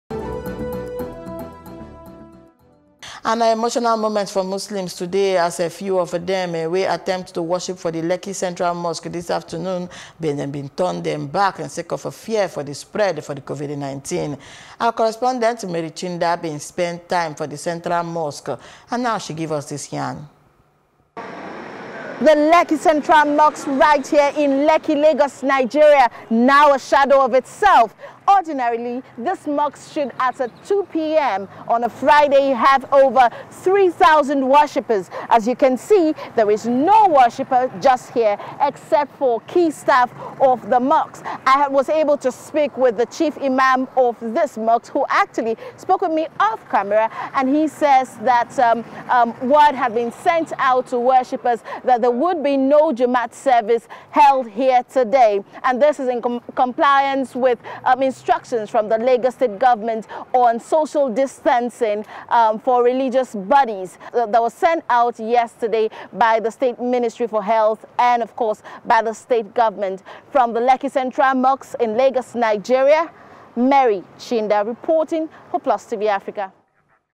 Covid19: Lagos Jummat Service Cancelled
Na emotional moment for Muslims today as a few of dem wey attempt to worship for the Lekki Central mosque this afternoon been dey turned back sake of fear for the spread of the Covid-19.